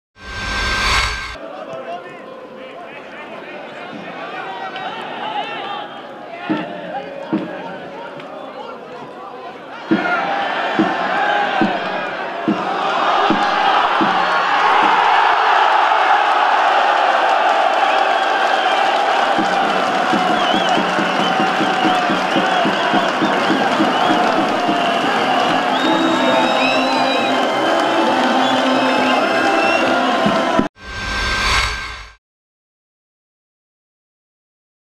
Petneházi Márk gólja a Mezokővesd Zsóry FC - Puskás Akadémia FC mérkőzésen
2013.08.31. OTP BANK LIGA 6. forduló
Mezokővesd Zsóry FC - Puskás Akadémia FC 3-1
Petneházi Márk gólja